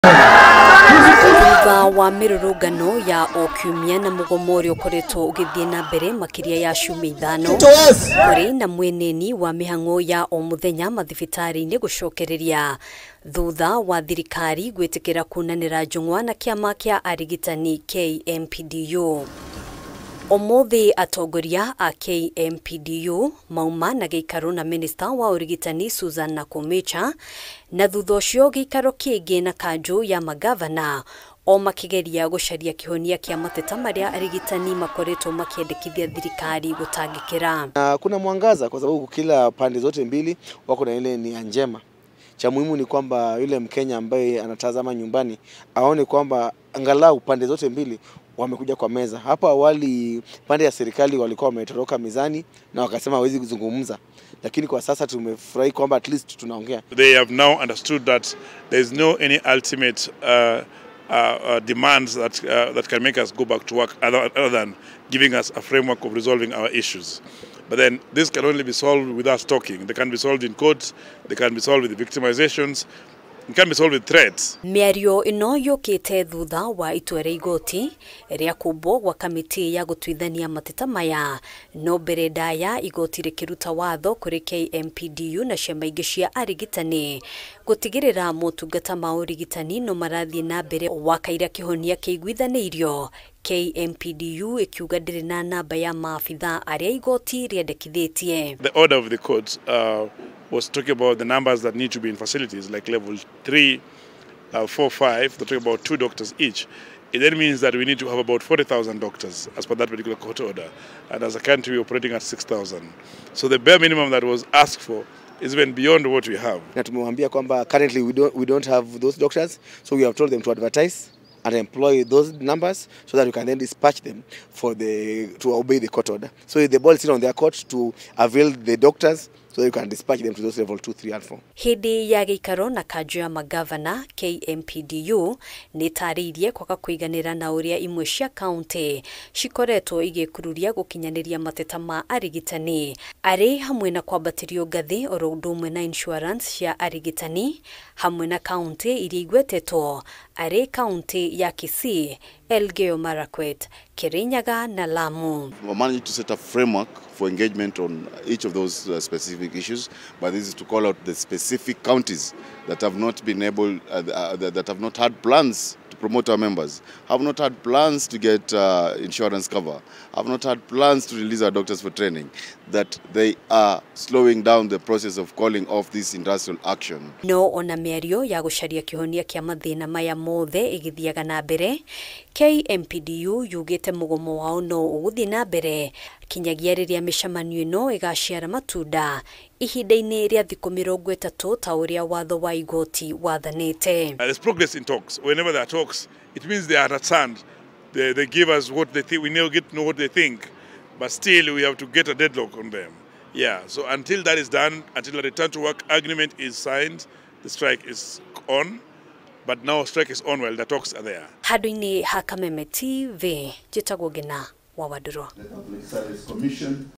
KMPDU gucemania na minista Nakhumica na kanju ya mangabana
KURI NA MWENENI WA KURUGAMIO KWA MUGOMO WA MAABITHAA A URIGITANI URIA UKORETWO UGITHII NAMBERE MAKIRIA YA CIUMIA ITHANO, THUTHA WA THIRIKARI GUITIKIRA KUNANIRA NJUNG’WA NA KIAMA KIA ARIGITANI KMPDU  NIGUO GUCARIA KIHONIA
 ONA KUHANA ORO UGUO KMPDU YUGITE MUGOMO NO UGUTHII NAMBERE NGINYAGIA GUKAGIA NA NGIRIMITI YA GUTUMA MACOKE WIRA UMWE WA MAUNDU MARIA MAHINYIRIIRIE NO MUHAKA UTHURIMWO ARI KUONGERERWO  KWA NAMBA YA ARIGITANI